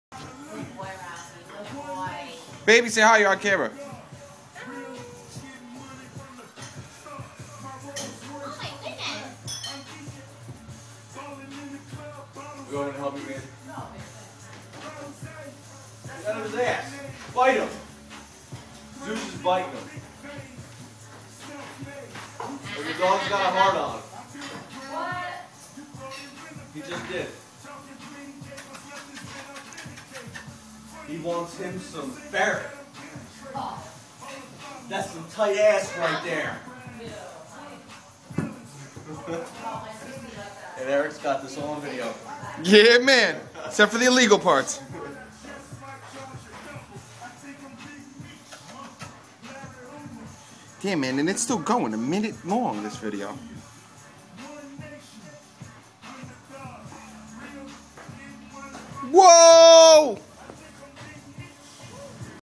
Golden retriever plays with ferrets
Our golden retriever ralphie plays with a ferret.